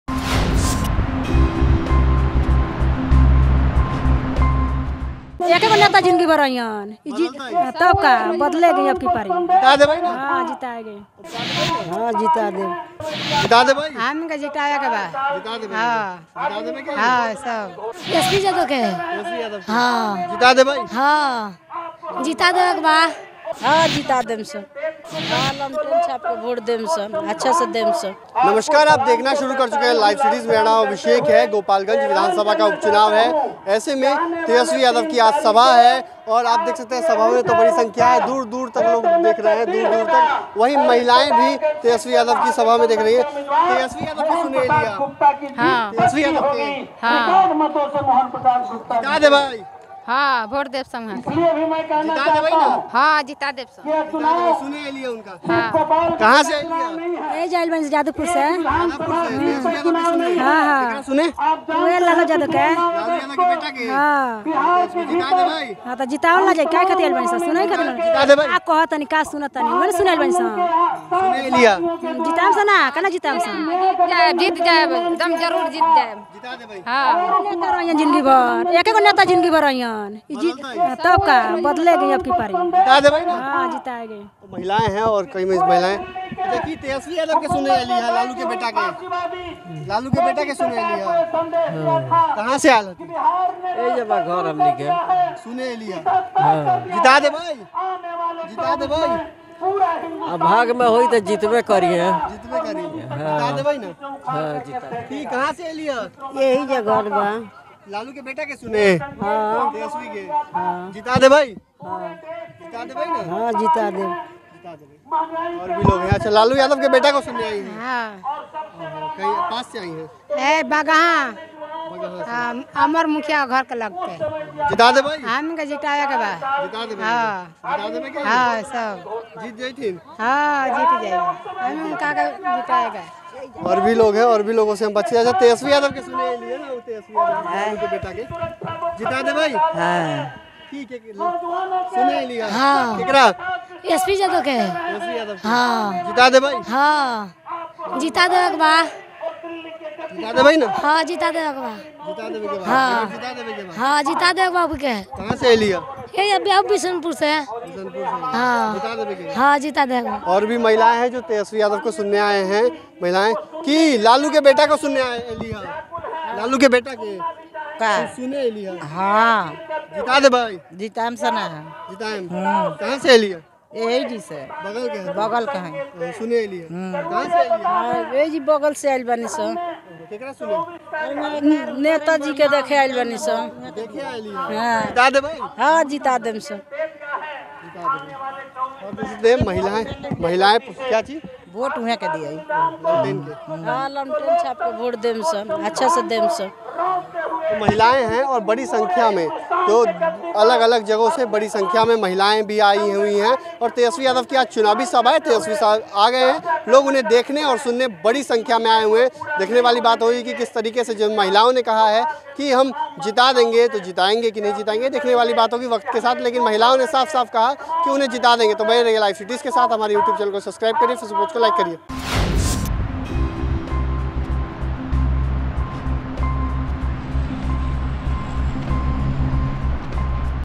Gopalganj Chunav : Tejashwi Yadav की सभा में आईं महिलाओं को सुनिए, Lalu Yadav पर भी बोलीं
Gopalganj Chunav : Tejashwi Yadav की सभा में आईं महिलाओं को सुनिए, Lalu Yadav पर भी बोलीं | Bihar News | LiveCities